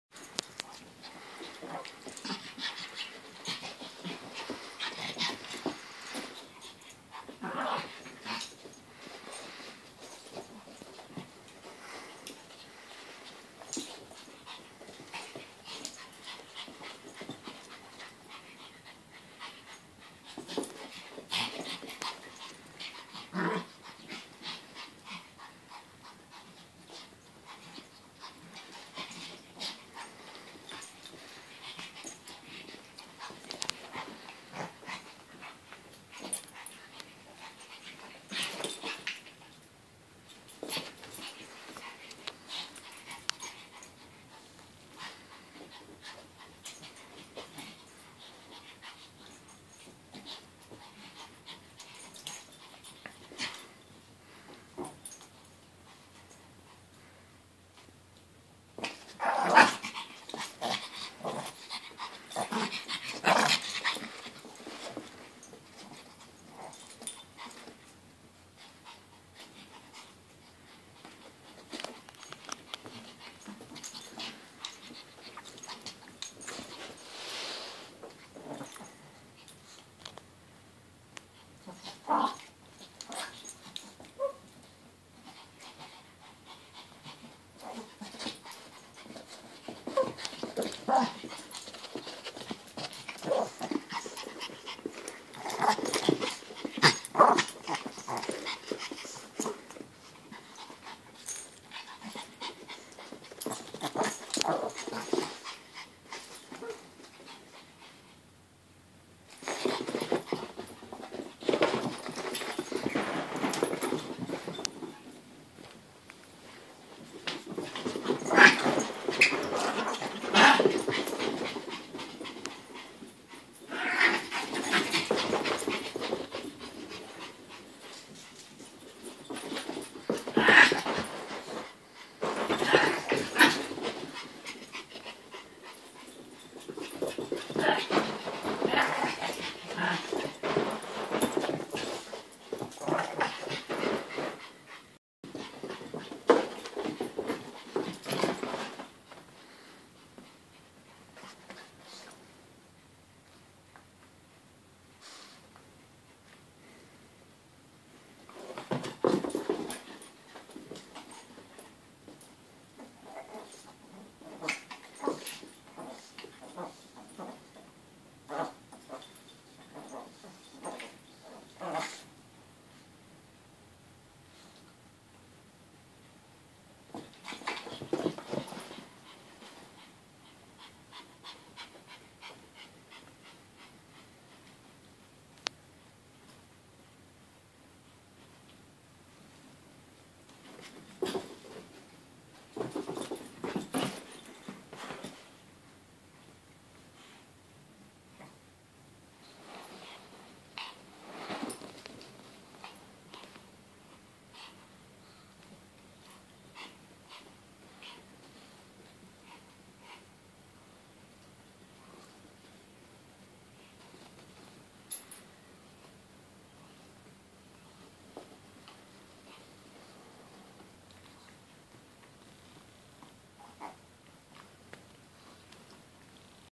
Using up energy
Khan started playing with Ronin until hes out of energy and they sleep on the couch.